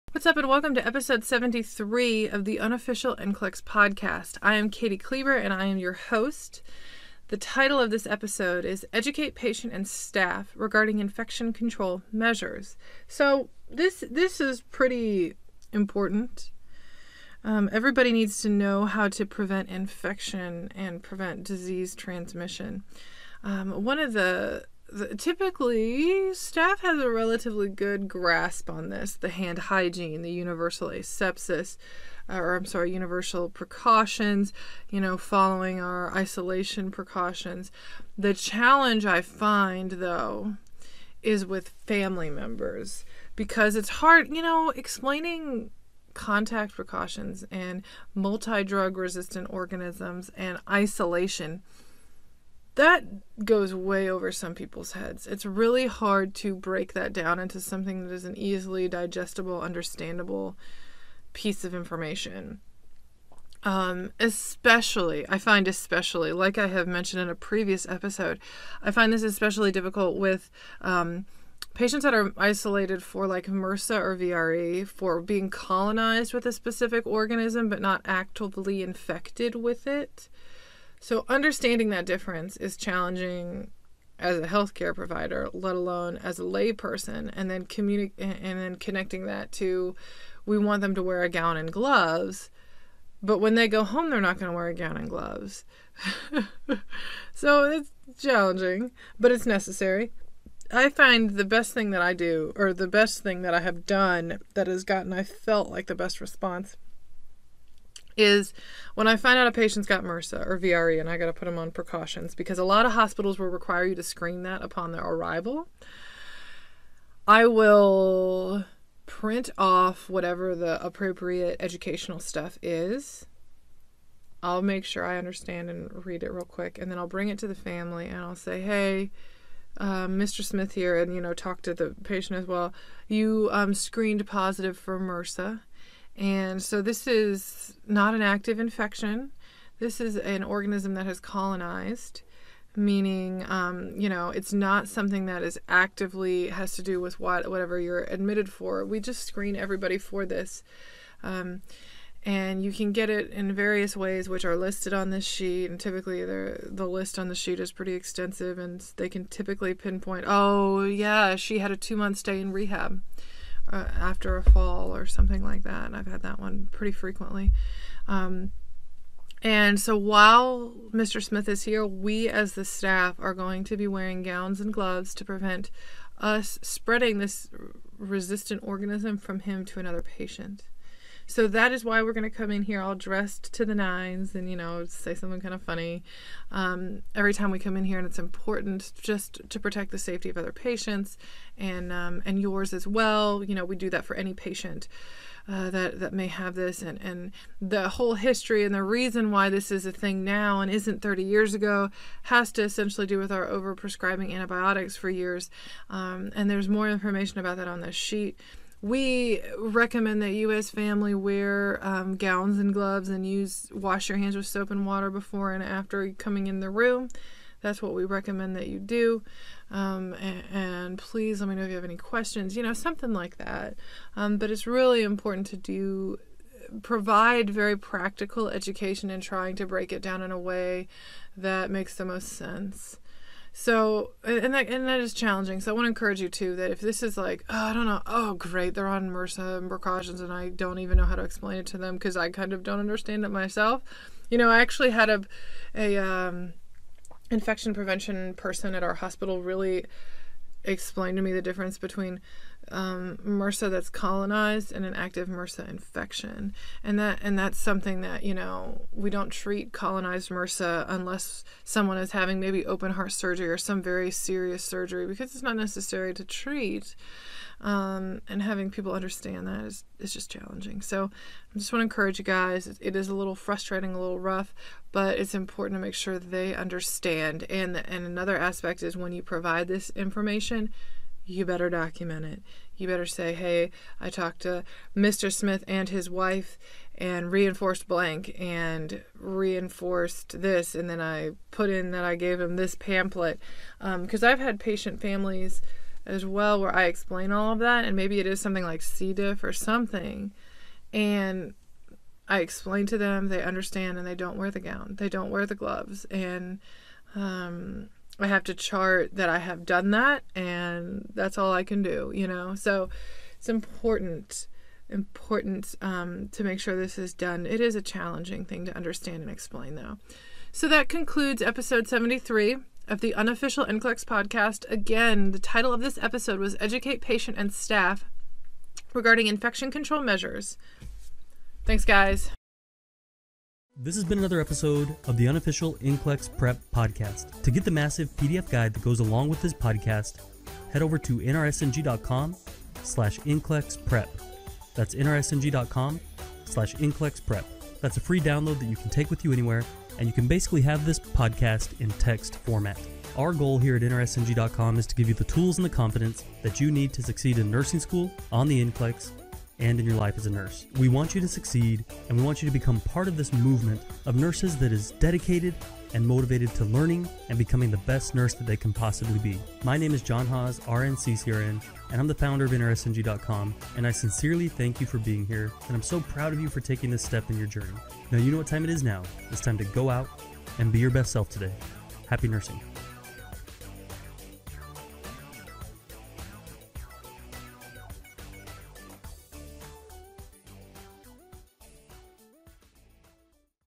Ep073 Safety and Infection Control Standard Precautions (NCLEX Prep Podcast)
Really important, especially with first time parents
Can be easily done incorrectly, not as straightforward as some anticipate
Some facilities may have it as part of policy to make sure family knows how to properly use before leaving